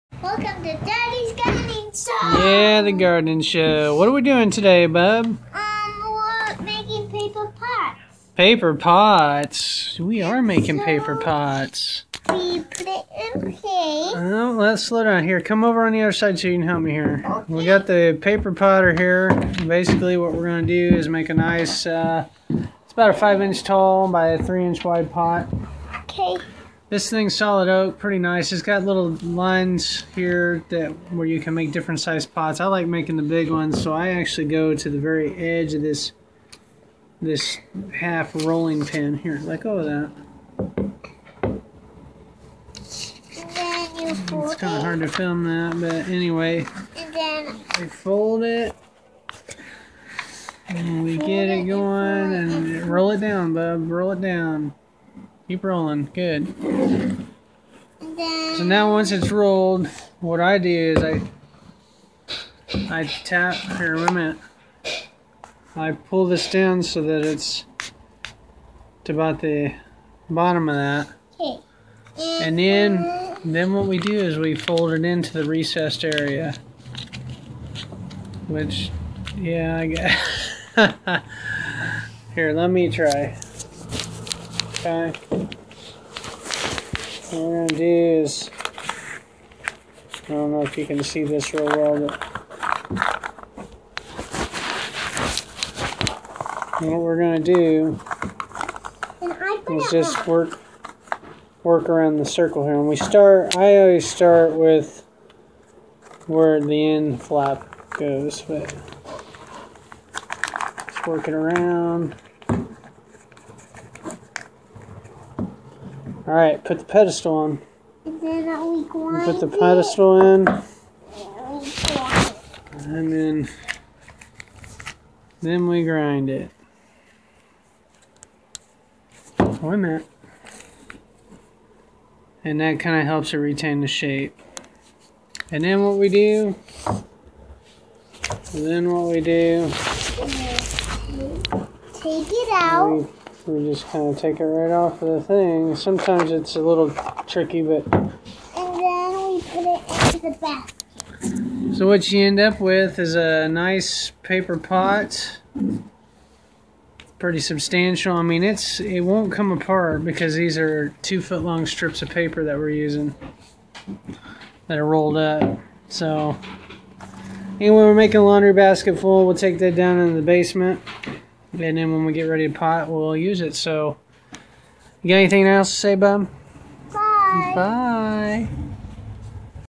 The Big Paper Potter
I found this tool while cruising around the Greenhouse Megastore  website.   It's been pretty handy so far.  We'll see how the pots hold up.